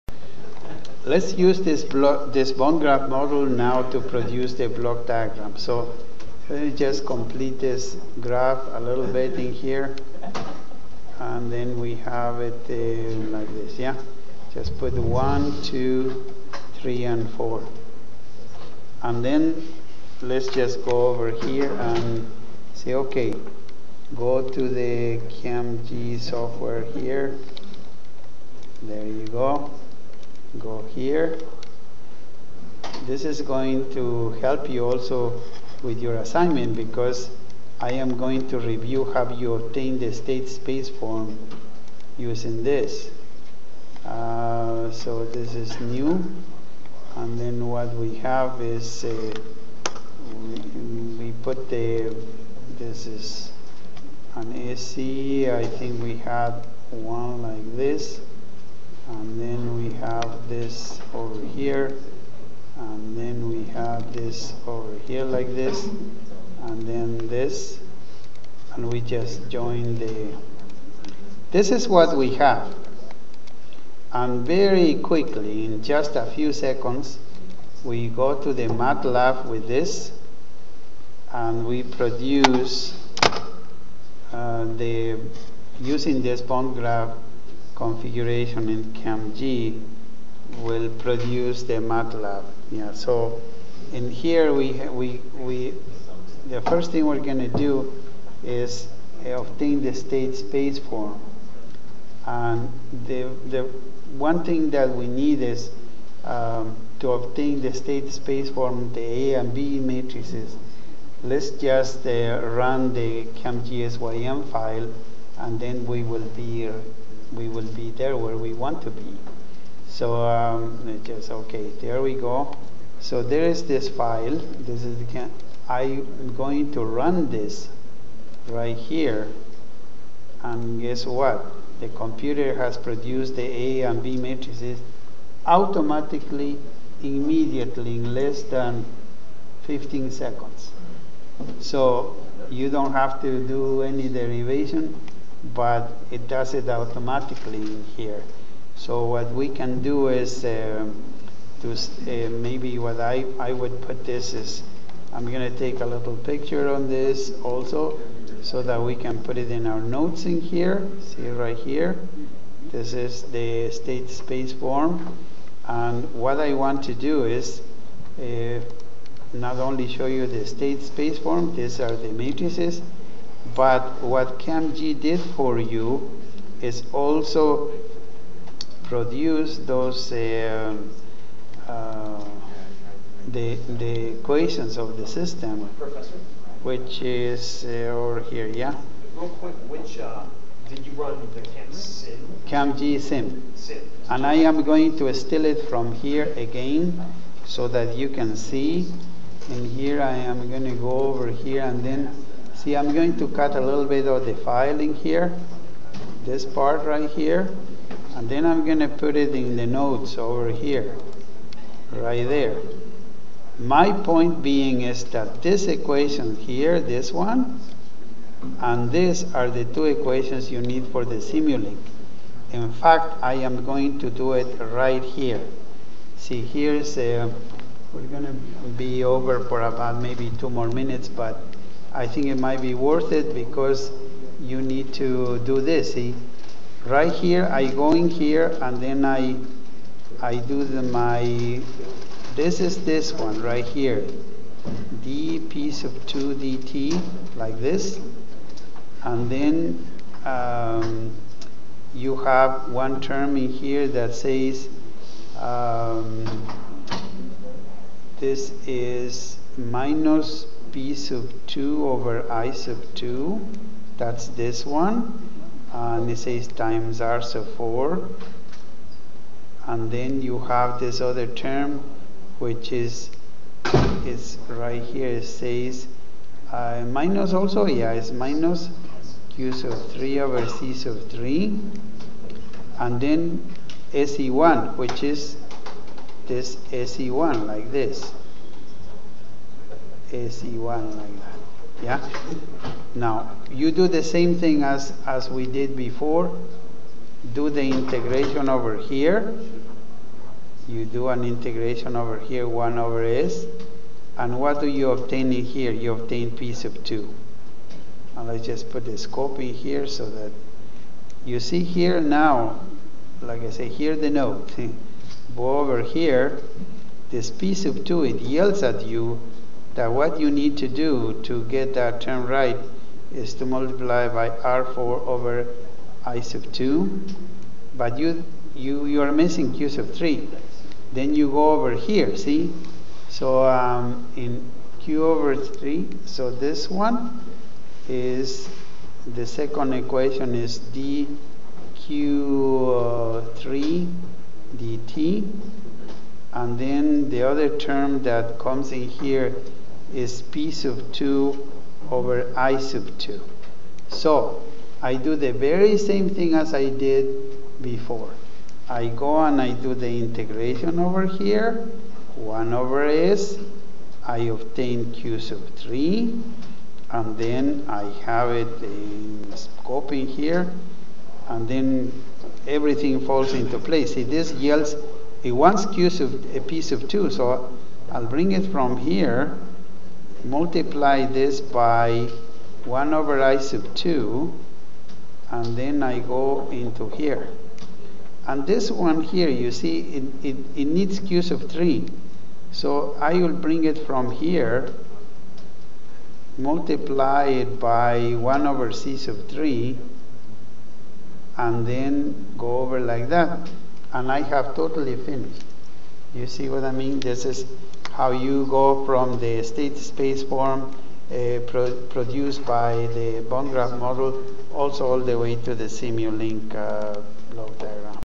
Mechanical example (From Bond Graph Model equations to Block Diagrams) 25102012_1020_30
Using a mechanical system see how you can derive the first order equations using CAMPG and then generate a block diagram.  This exercise is to practice converting first order equations into Simulink block diagrams.